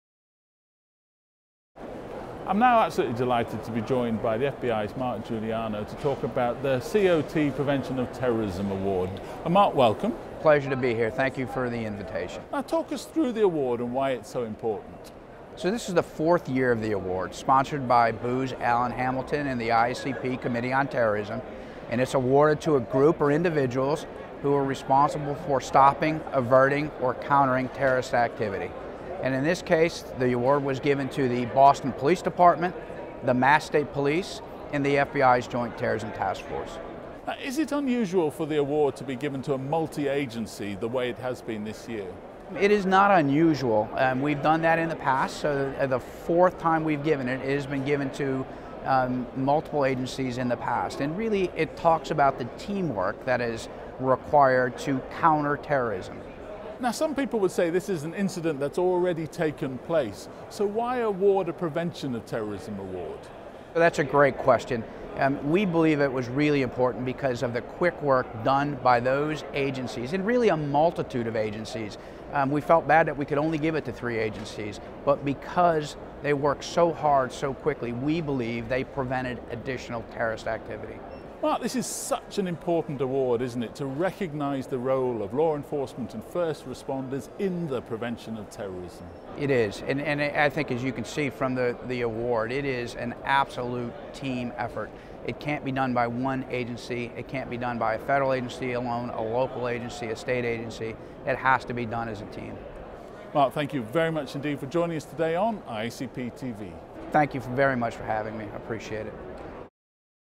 Collaborative Efforts to Reduce Terrorism - IACP Committee on Terrorism
IACP TV talked to Mark Giuliano, Chair of the IACP Committee on Terrorism (FBI SAC, Atlanta), about the tremendous collaborative efforts in the aftermath of the Boston Marathon Bombings involving the Boston Police Department, the Massachusetts State Police, and the Federal Bureau of Investigation.